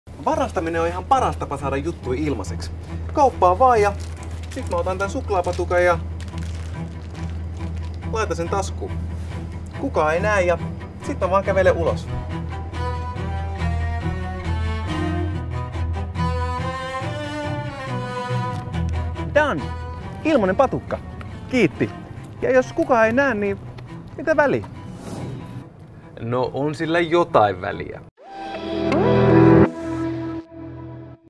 Mun elämä, mun vastuu -oppitunnit – toisen omaa ei saa ottaa
Mun elämä, mun vastuu on opetussuunnitelmaan perustuva, viiden oppitunnin oppimiskokonaisuus kolmannelle luokalle, jossa käsitellään vastuullista toimintaa ja oman talouden hallintaa.

Oppituntien aikana oppilaat tutustuvat yhteiskunnan toimintaan, rohkaistuvat toimimaan vastuullisesti aktiivisena osana yhteiskuntaa, oppivat oman talouden hallintaa, hahmottavat yritysten toimintaan liittyviä käsitteitä sekä harjoittelevat ympäristön kannalta kestävällä tavalla toimimista.

Oppitunnit löydät TATin sivustolta.